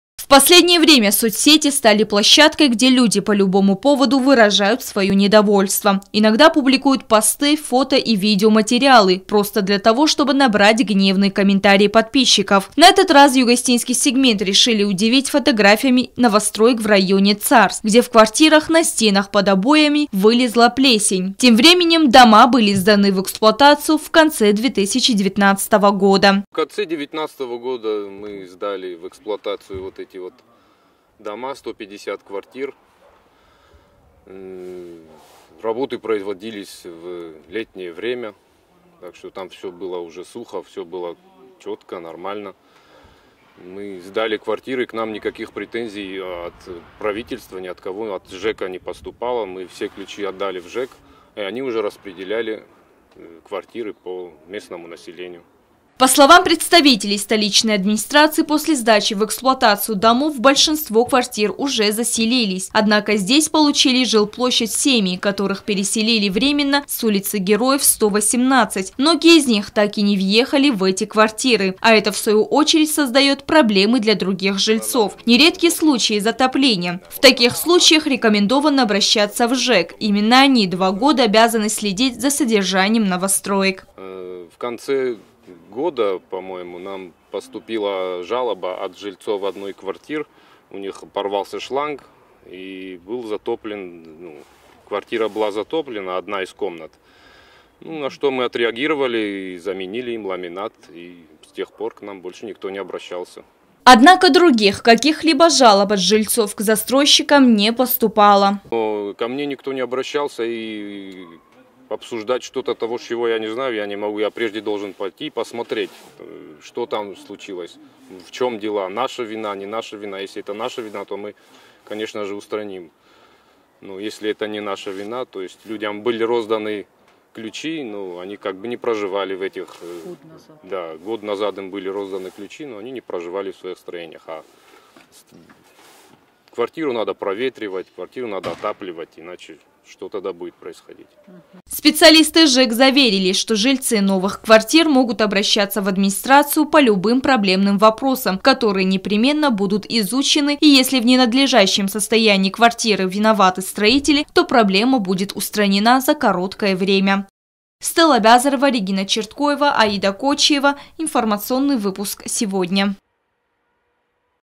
За последние годы в республике было построено немало домов
Многие семьи получили возможность улучшить свои жилищные условия. Но иногда новоселы могут столкнуться с определенными проблемами – например, недочетами строительства. В таком случае следует обращаться в ЖЭК при администрации города.